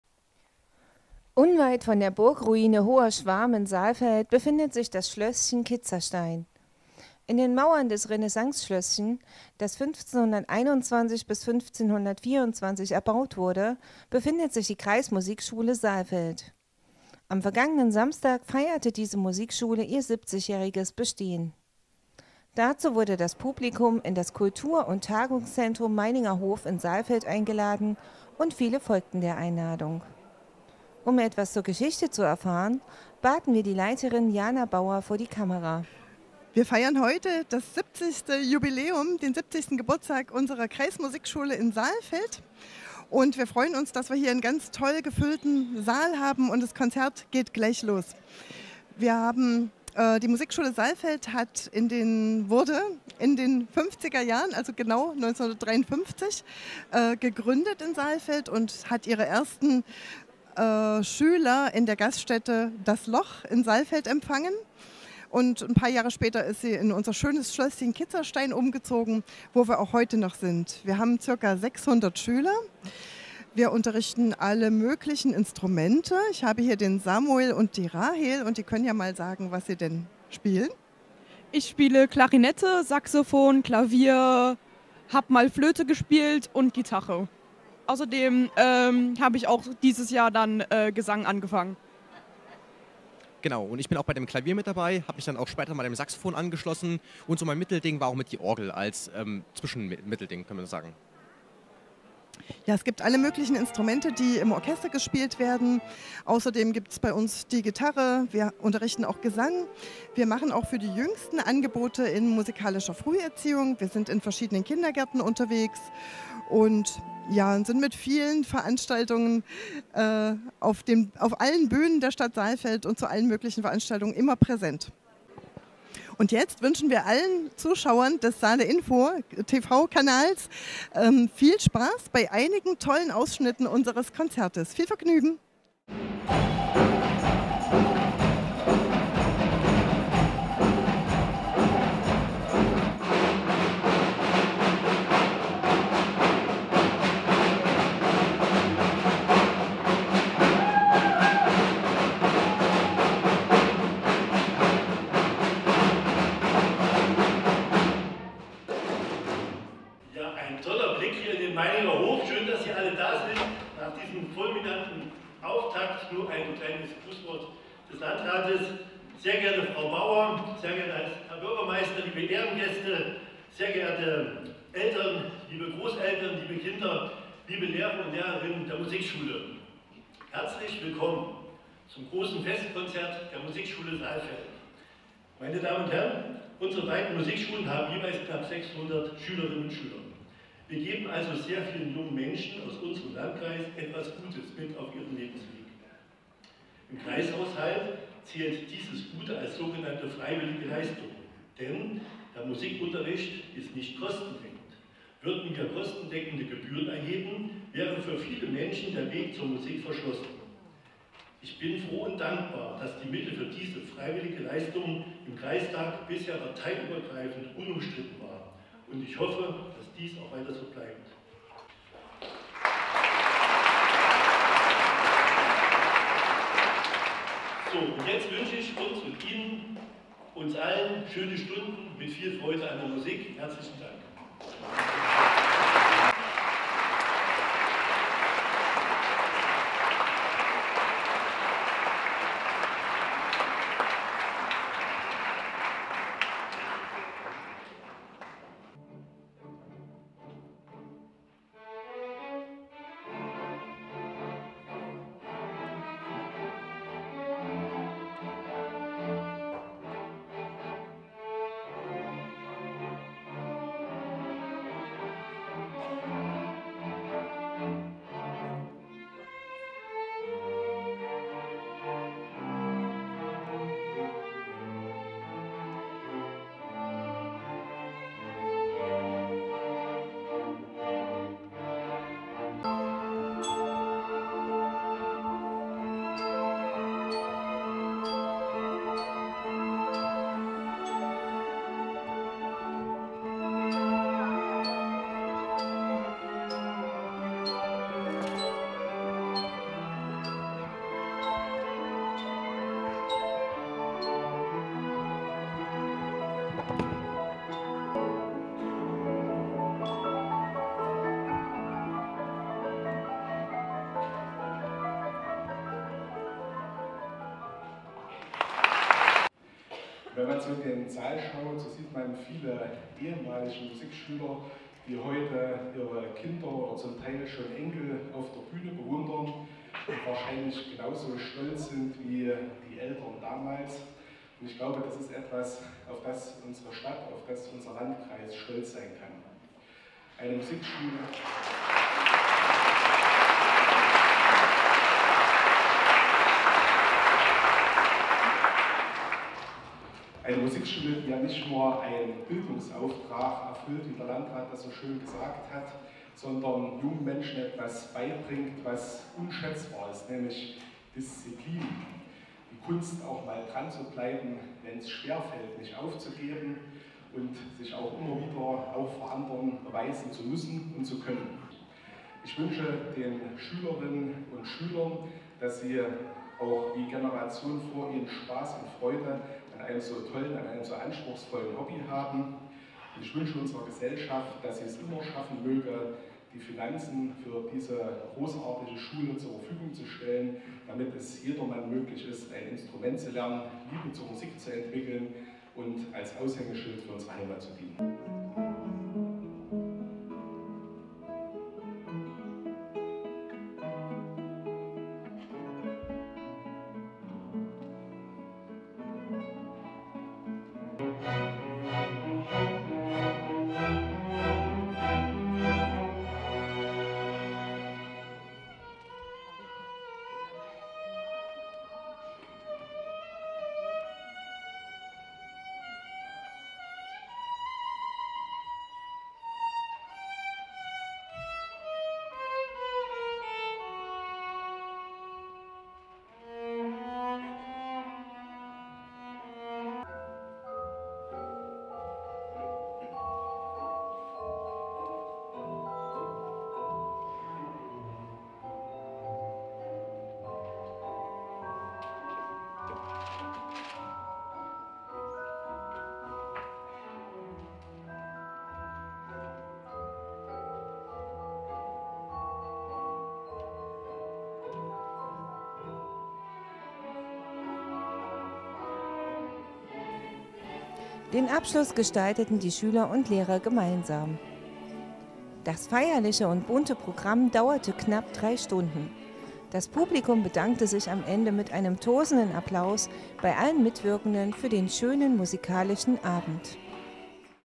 70 Jahre Kreismusikschule Saalfeld. Das festliche Konzert findet im Meininger Hof in Saalfeld statt
Dazu wurde das Publikum in das Kultur und Tagungszentrum Meininger Hof in Saalfeld eingeladen und viele folgten der Einladung.